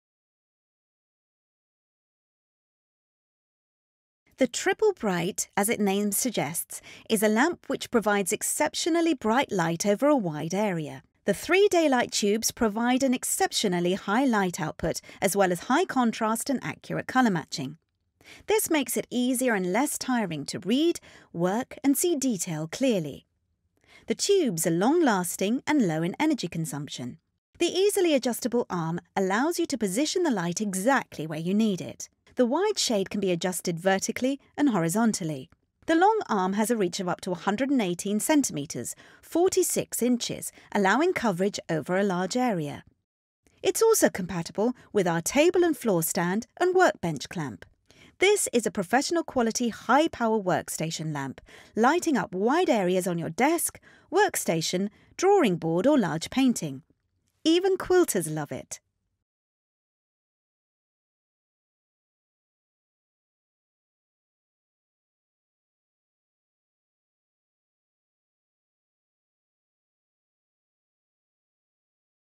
Daylight Lighting: Triple Bright Lamp
The Daylight Lighting: Triple Bright lamp is the brightest of the Daylight range. It is great at producing the maximum amount of light. With 250w of pure white light, this is a professional high-power workstation lamp, lighting up wide areas on your desk, work station, drawing board or large painting. The lamp has 3 x 14w energy saving Daylight™ tubes offering an incredible 42w pool of light. The 60cm large shade can be smoothly adjusted vertically, horizontally and laterally to any desired position. With this light you will be surprised by how much better you will see the finer details and colours in your work. It is also widely used by printers to check printwork and books and by jewellers to inspect precious stones and gems. The low heat full spectrum Daylight™ light with 6,500°K improves contrast, reduces glare and is of the highest quality for colour rendering.
Measurements: Lamp neck to base – 80cm; Power cord – 190cm.